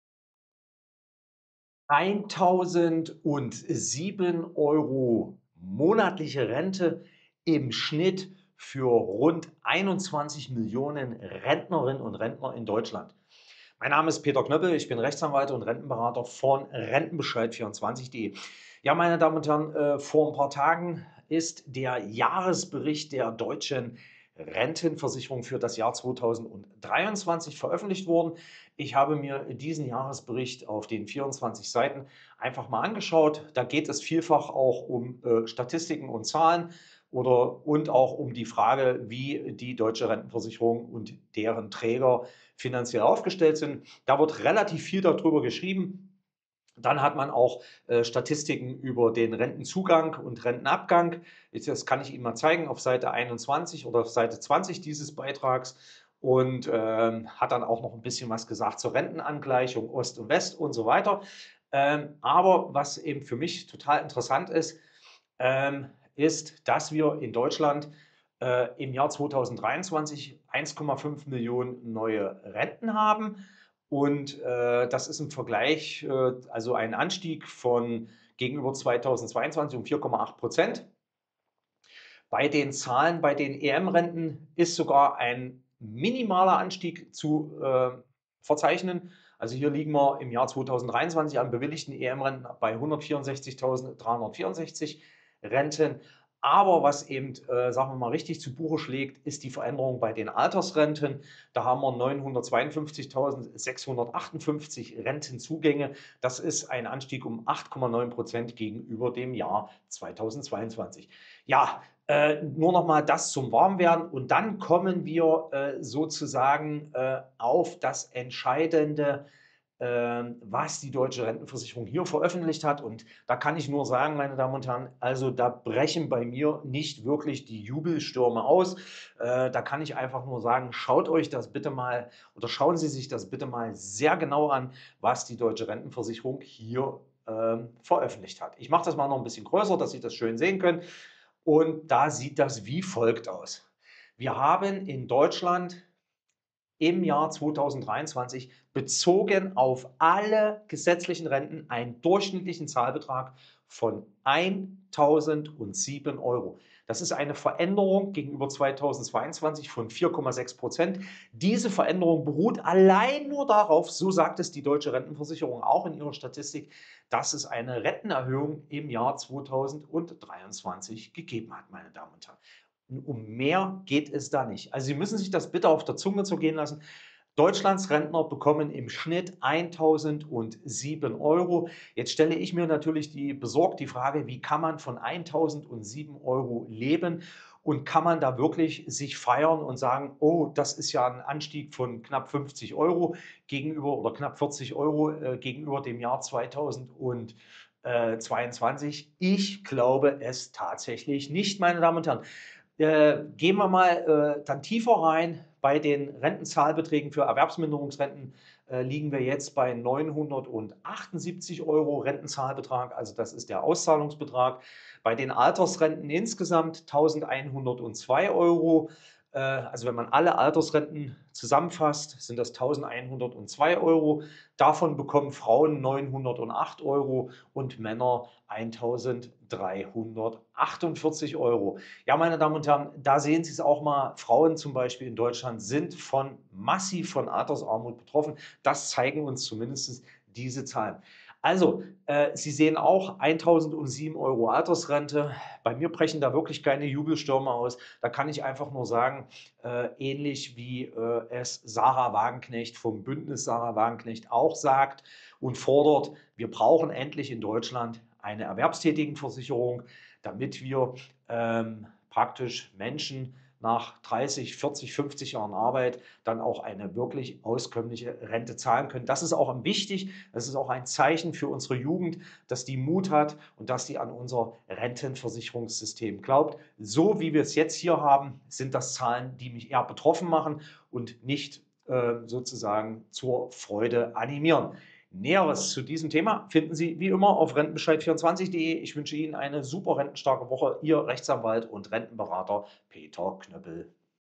Rente nahe der Grundsicherungsgrenze: 1007€ Monatsrente im Schnitt für 21,2 Millionen Rentner
Jedes Jahr erscheint der Jahresbericht der Deutschen Rentenversicherung. Viele Zahlen´, Daten und Fakten präsentiert die Rentenversicherung. Es geht um die Finanzlage der Rentenkasse aber auch um wichtige Informationen darüber, wie viele neue Rentner es gibt und wie hoch die Renten im Schnitt ausfallen. Eine Zahl fällt auf, die alarmierend ist. Sie lässt Rückschlüsse darauf zu, in welchem Zustand sich unsere Rentenversicherung befindet und welche Auswirkungen dies auf die Zukunft der Rente hat. Welche Zahl genau dies ist, hier in diesem Video berichten wir davon.